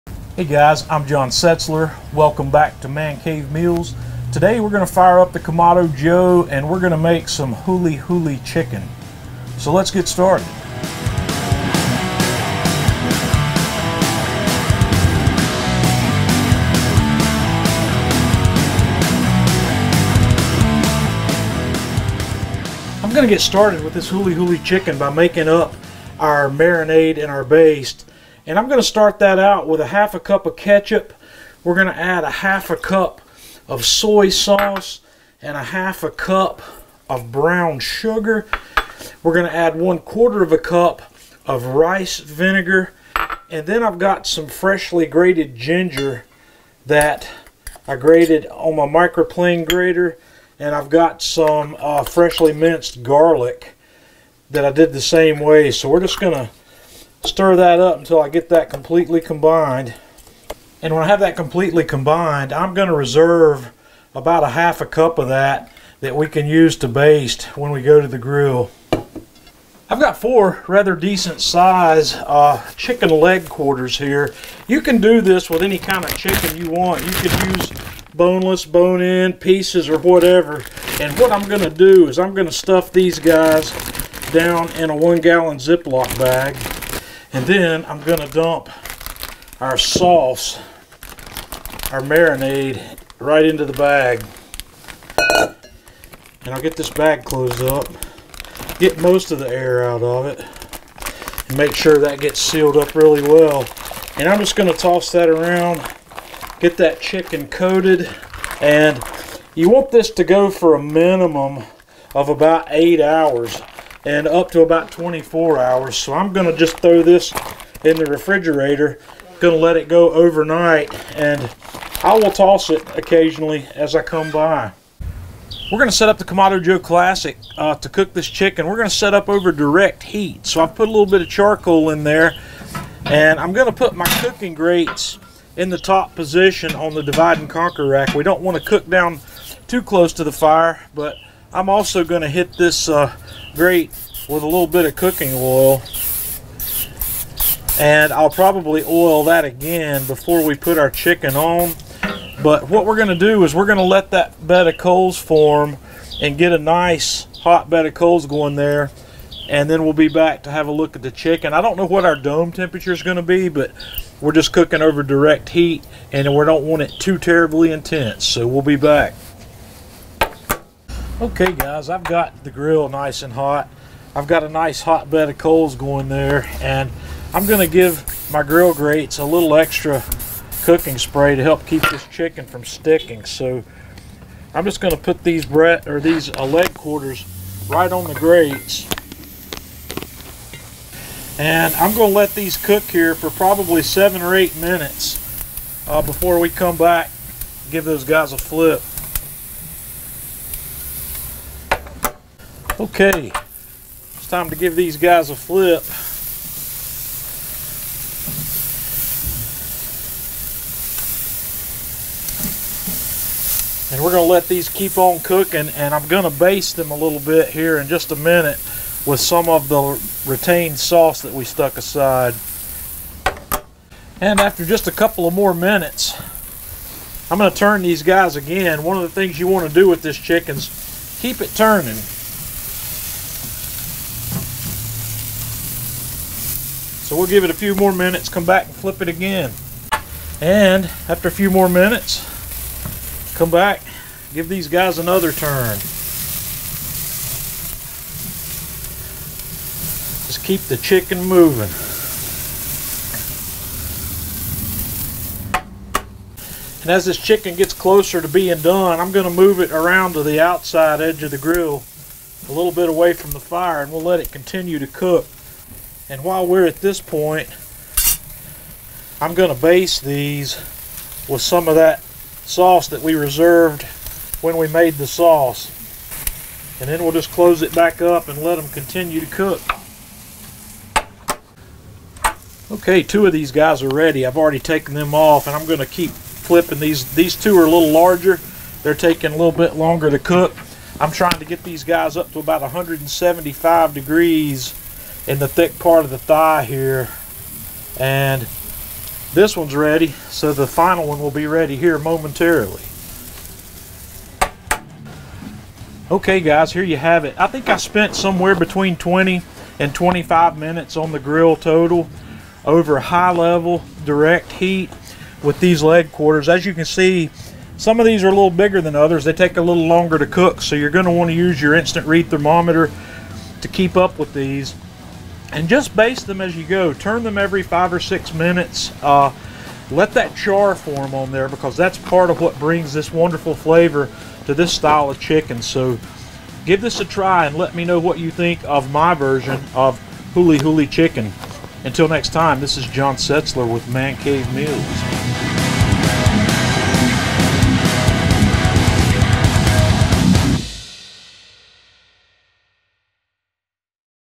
Huli Huli Chicken
Every now and then you just wanna toss some chicken pieces on the grill and have a great meal with it.  I decided to whip up some Huli Huli Chicken and do a very simple direct heat cook to make some amazing chicken leg quarters on the Kamado Joe Classic 3 grill.  You can use this marinade/glaze recipe on any kind of chicken you like.  You can use bone-in or boneless pieces as well.  Here's the recipe for the marinade and glaze:

1/2 cup ketchup
1/2 cup soy sauce
1/2 cup brown sugar
1/4 cup rice vinegar (or cider vinegar)
2 to 3 finely minced cloves of garlic
2" piece of ginger, peeled and finely minced

Reserve about 1/2 cup of this sauce to use as a glaze while the chicken is on the grill.

Set your grill up for direct heat cooking with the grill grates in the highest position.  Get a nice hot bed of coals going in the fire box, but don't load a LOT of charcoal.  Oil your cooking grates and then put the chicken pieces on.  Let them char to your own taste and then turn them and keep turning them frequently until they are done.  I like to cook my dark meat pieces to an internal temperature of about 175°F and I would pull breasts off at 150-155°F.